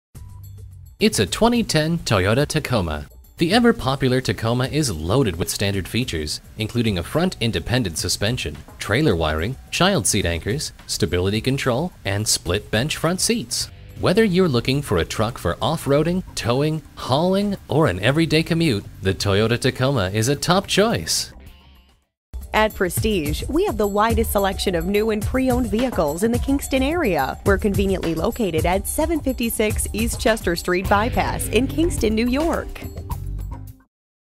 2010 Toyota Tacoma Kingston NY Poughkeepsie, NY
Year: 2010
Make: Toyota
Model: Tacoma
Trim: Base
Engine: 4.0L V6 SMPI DOHC
Transmission: 5-Speed Automatic with Overdrive
Mileage: 39791
Stock #: AZ71307A
VIN: 5TEUU4EN1AZ713072
You won't find a better truck than this great 2010 Toyota Tacoma. It is nicely equipped with features such as Convenience Package Option 1 (Cruise Control, Off-Road Grade Package (Bilstein Shocks, Front Tow Hooks, Metallic Trim Instrument Panel, Off Road Tuned Suspension, 115V/400W Deck Mounted Power Point, 16" x 7J+30 Aluminum Disc Wheels, and 4.0L V6 SMPI DOHC.

Address:
756 East Chester St By-Pass